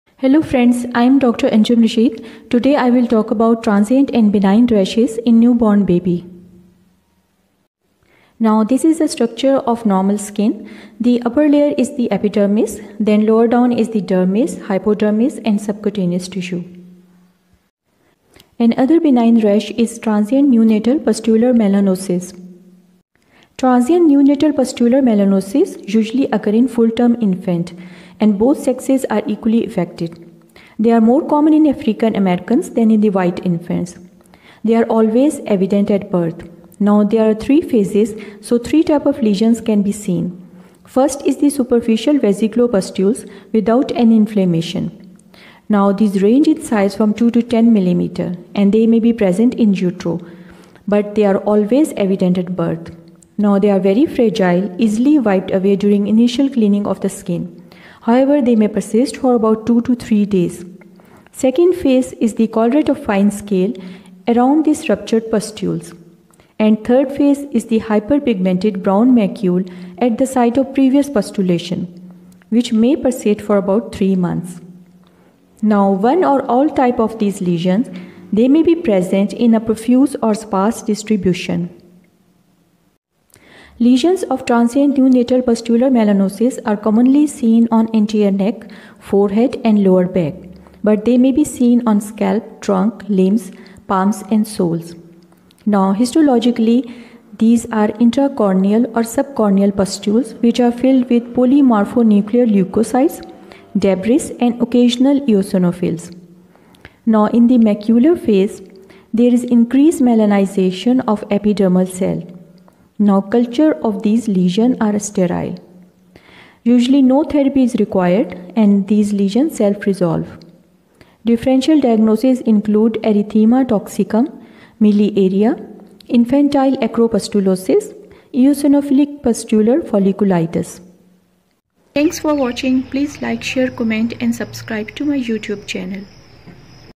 Newborn Baby Rash - Part 2 | Transient Neonatal Pustular Melanosis Cause, Symptoms, & Treatment
This video is about Newborn Rash -Transient Neonatal Pustular Melanosis. It is also called Pustular Melanosis. It is a transient rash in newborn babies. It is a benign harmless skin condition. The rash occurs only in newborn period. The lesions are present from birth. They usually begin as 2-3 mm vesiculopustules or blisters that are not surrounded by any erythema or redness. These blisters easily rupture leaving pigmented macules or spots with surrounding collarette of scales. Blisters usually resolve in 2-3 days. The remaining dark spot eventually fade away in 2-3 months. Lesions may occur singly or in groups. Transient neonatal pustular melanosis can occur on any part of the newborn's body but typically on forehead, chin, behind ears, neck and back. Palms and soles may also be affected. These lesions are benign and generally no therapy is required.